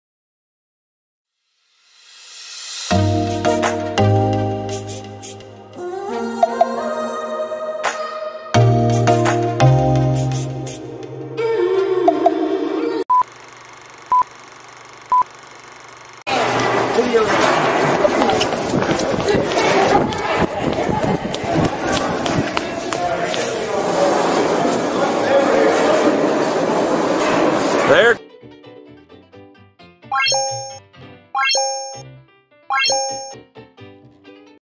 Ernie Shell arrest from "STIFFING" wrestlers as a shady Promoter!
"It's all a movie, nothing is real. and. it will end badly"~Ernie Shell

Socials:
TWITTER: @SeylaVieToMeYT (I don't post often)